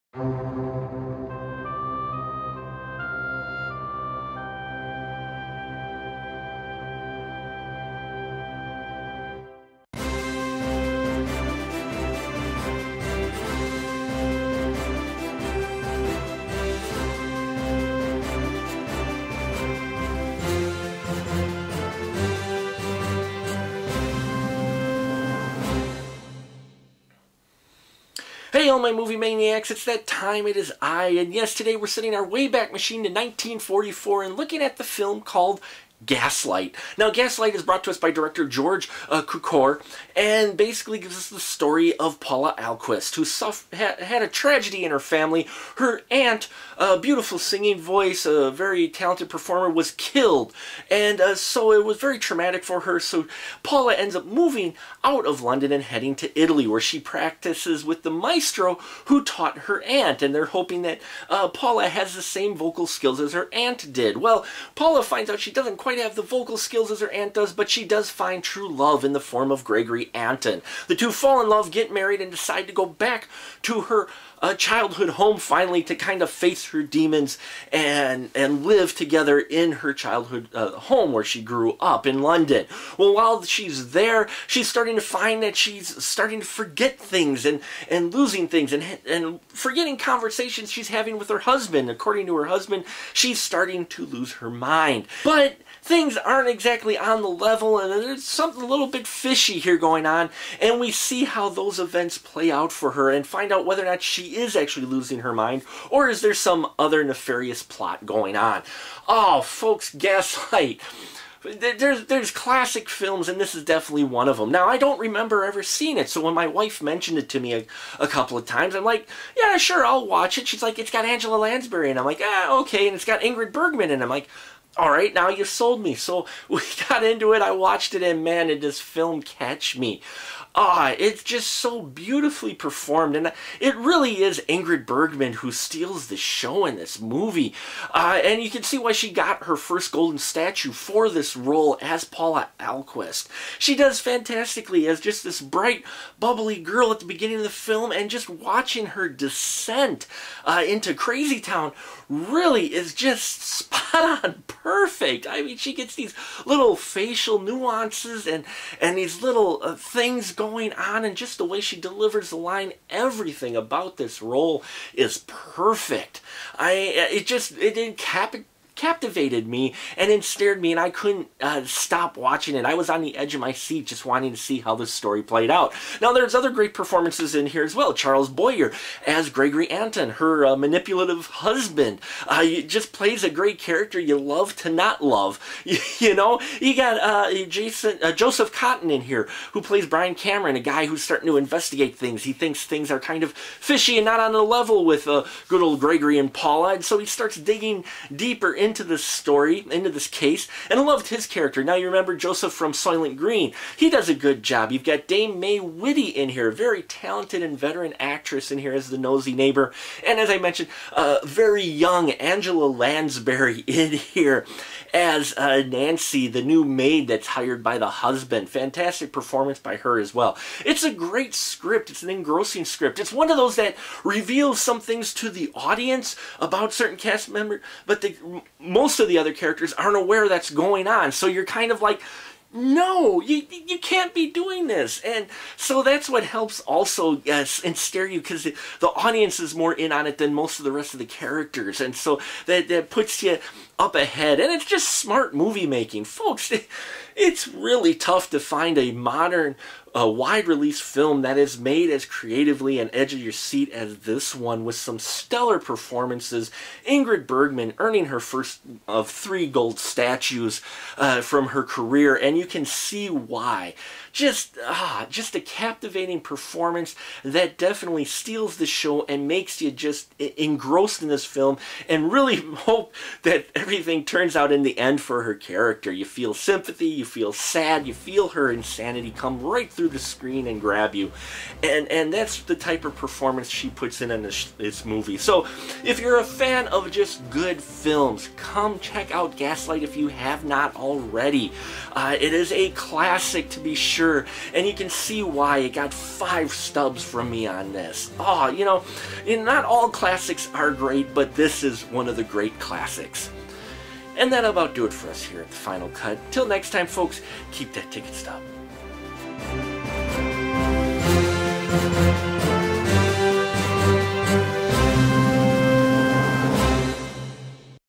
Gaslight (1944) review on The Final Cut
So my wife showed me something I don't remember seeing before and that is the movie Gaslight, a classic film from 1944.